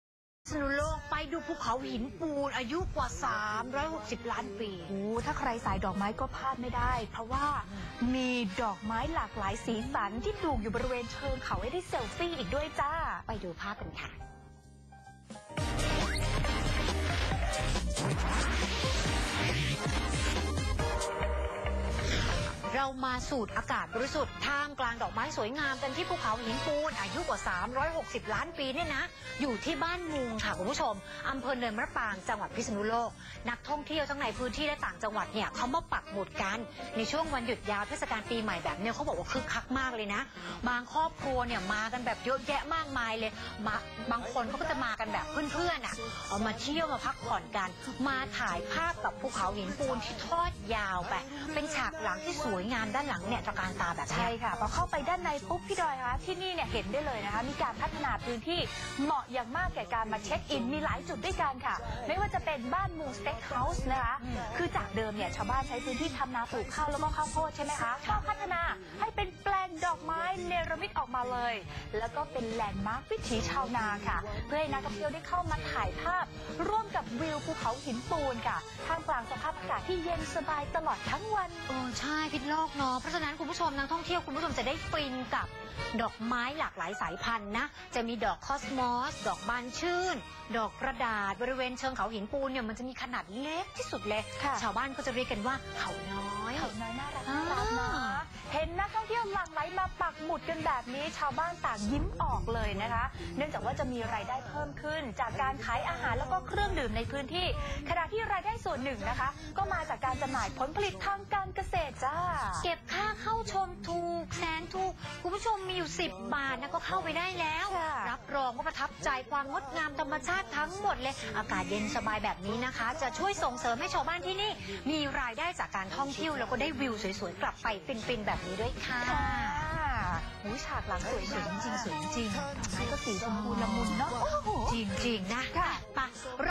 นทท.แห่ชมภูเขาหินปูน 360 ล้านปี คึกคัก!! | เรื่องดีดีทั่วไทย
นทท.แห่ชมภูเขาหินปูน 360 ล้านปี คึกคัก!! | 2/01/63 | ปักหมุดทั่วไทย | เรื่องดีดีทั่วไทย